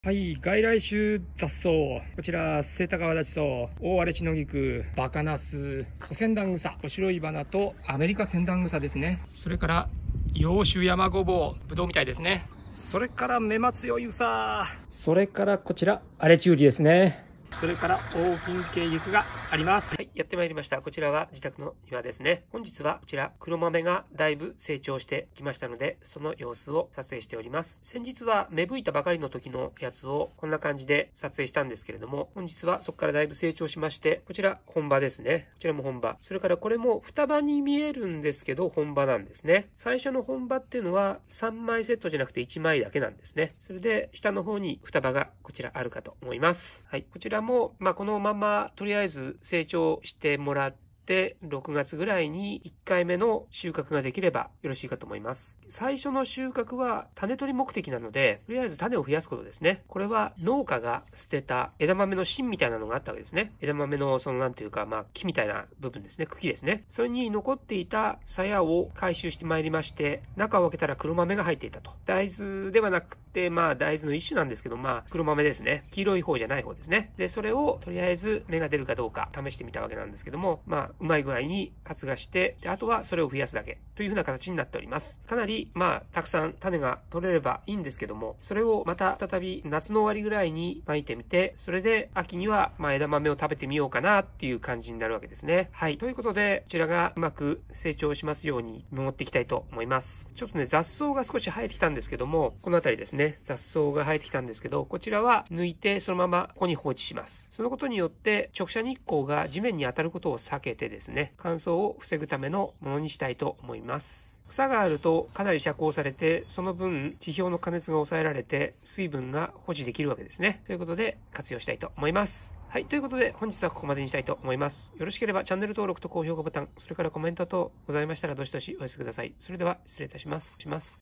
黒豆の本葉が出た外来種雑草灰の土地。
３月に蒔いた黒豆が、だいぶ大きくなってきました。

農家が廃棄したものがあり、それを回収しての栽培です。

肥料は、外来種雑草灰になります。